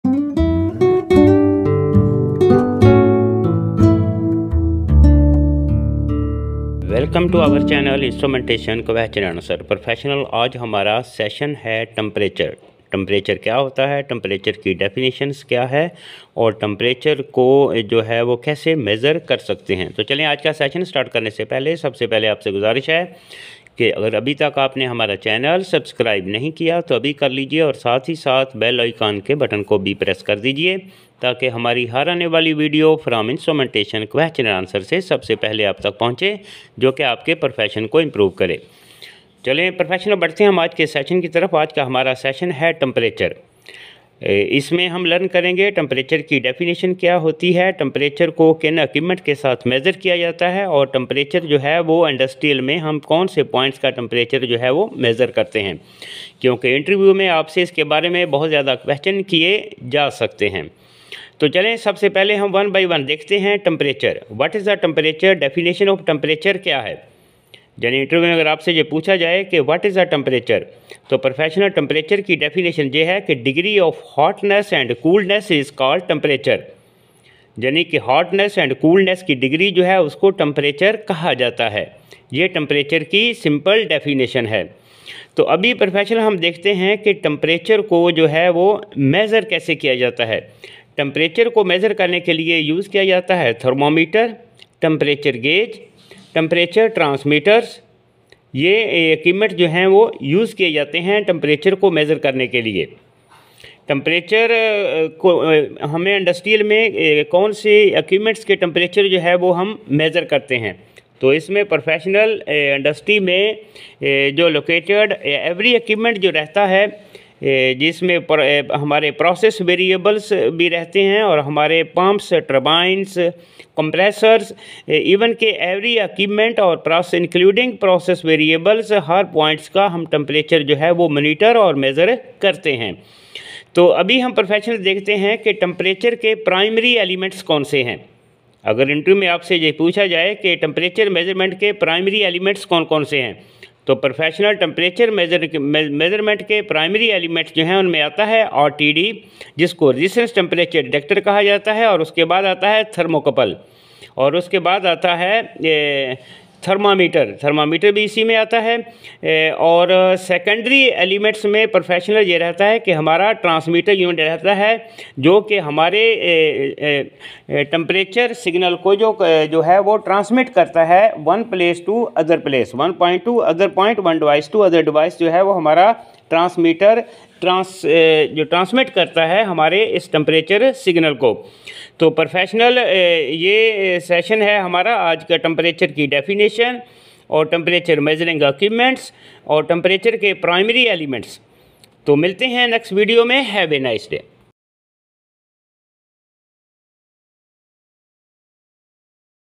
Definition of the Temperature And Measuring Elements
This video consists the detail of the Temperature Definition and measuring instruments in the different process etc